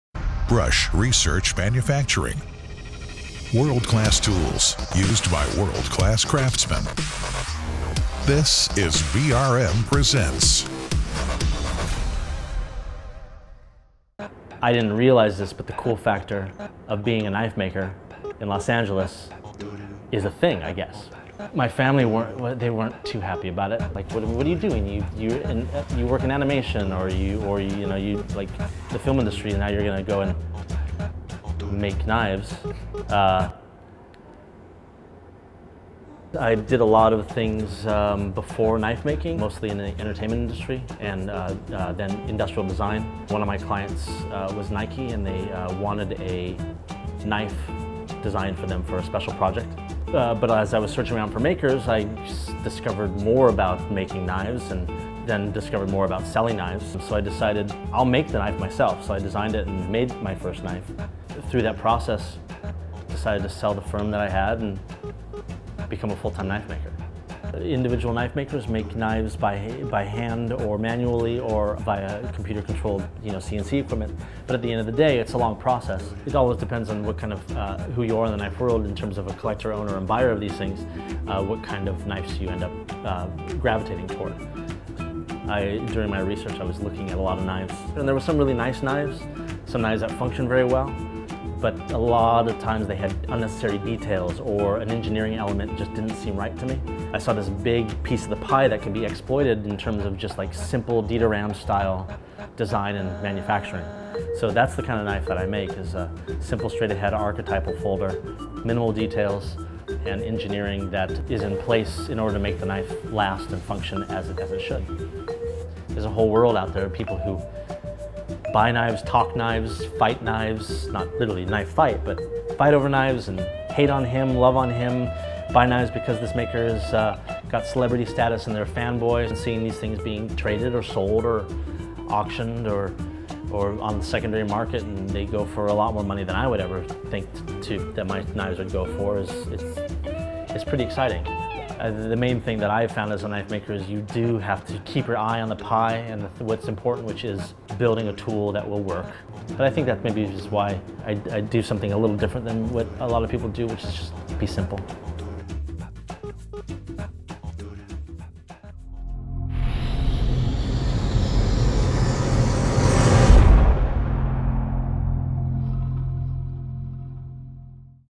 Why Balzano Knives Uses NamPower Brushes
Paulo Balzano is a professional knife-maker in Los Angeles. He could have been a film maker or an animator, but an industrial design project for Nike caused him to cut a new career path. Today, this American metalworker makes and sells knives with clean, straight lines and a distinctive look-and-feel. High-quality metalworking can be a “long process”, Balzano explains, but surface finishing and deburring are a lot smoother thanks to Brush Research Manufacturing (BRM).

As this video explains, Balzano Knives uses BRM NamPower technology to remove metal burrs and improve surface finish. NamPower nylon abrasive disc brushes come in two styles: dot and turbine. Both metalworking tools mount in CNC equipment for fast, efficient, repeatable operations. Dot-style NamPower disc brushes are ideal for light deburring and surface finishing. Turbine-style tools are the right choice for medium to heavy-duty surface finishing and deburring.

“You have to keep your eye on the pie,” Paulo Balzano says, “and what’s important – building a tool that will work.” For this world-class craftsman, world-class NamPower technology is the right choice.